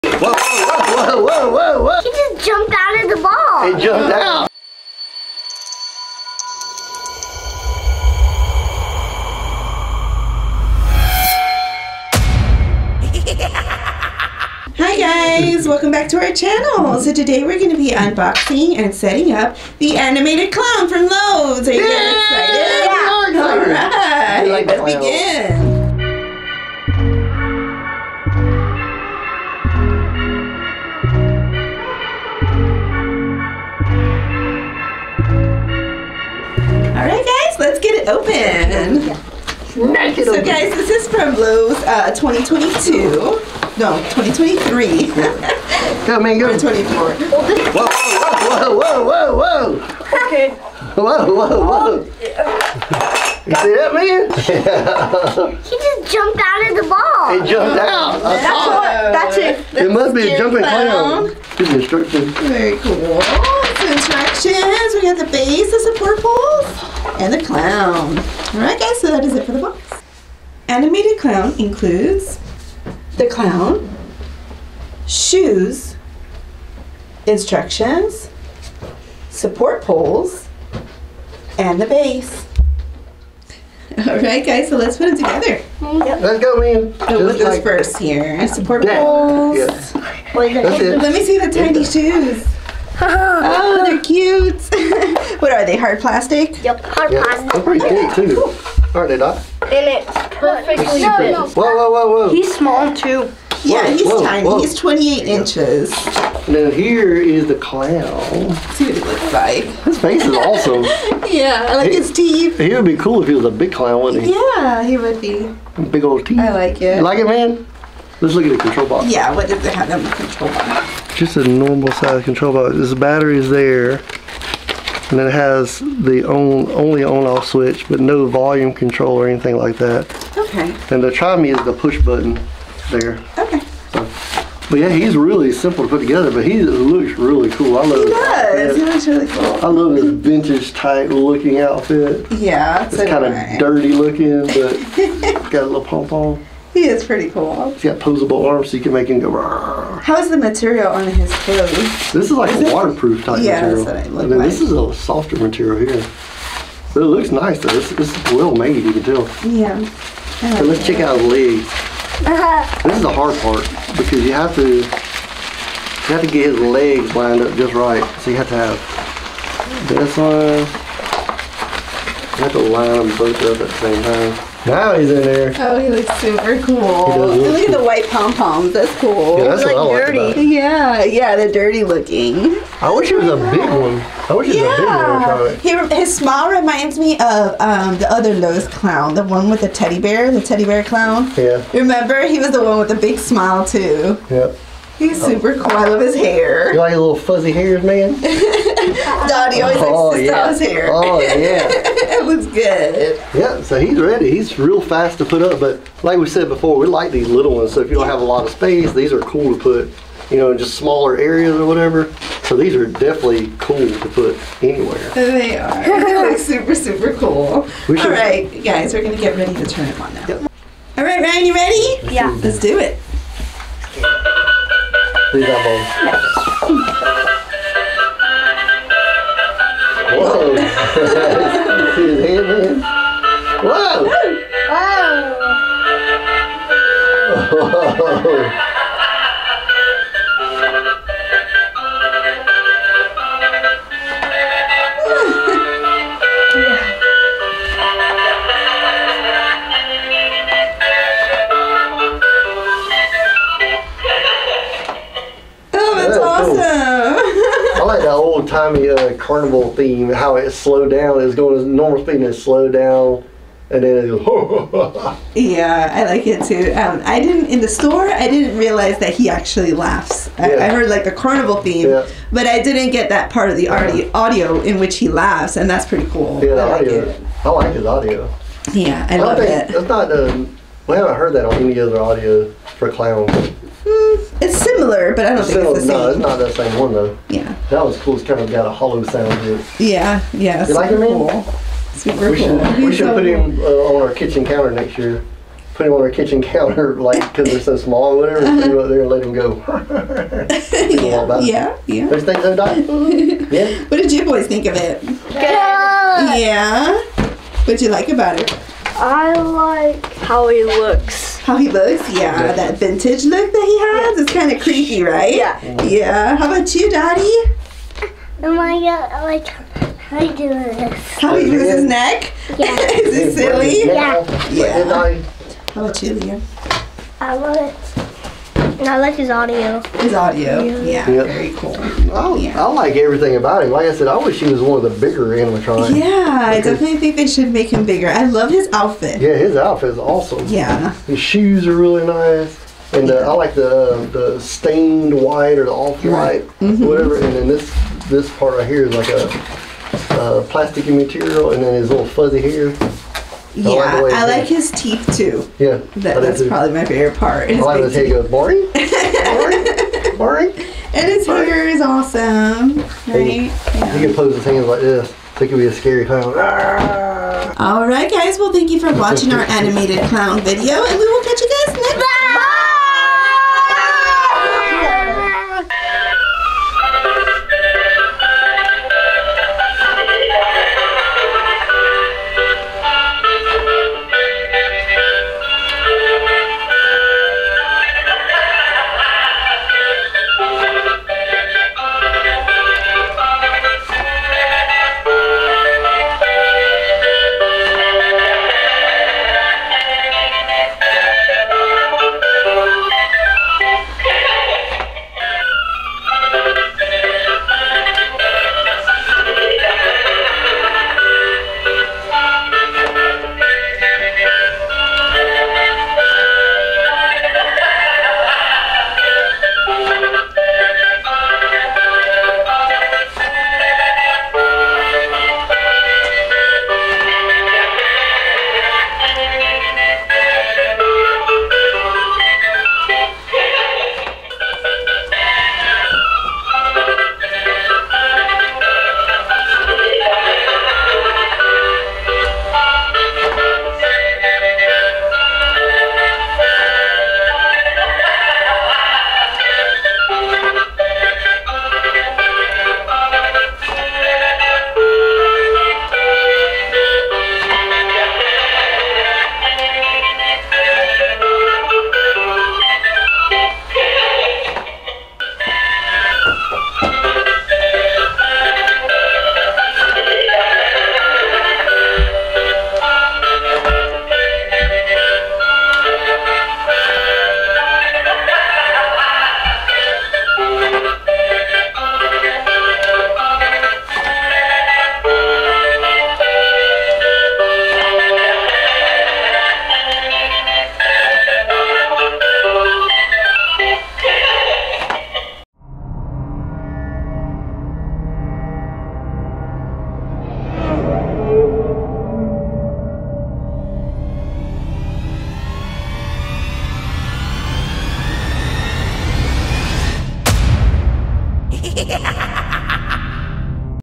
Animated Clown Lowes Halloween | Unbox Setup Halloween Animatronic | Creepy Clowns
In this video we unbox and setup Animated Clown from Lowes Halloween Animatronic lineup. This creepy clown Halloween animatronic moves it's neck side to side as it's eyes light up and scary circus audio can be heard.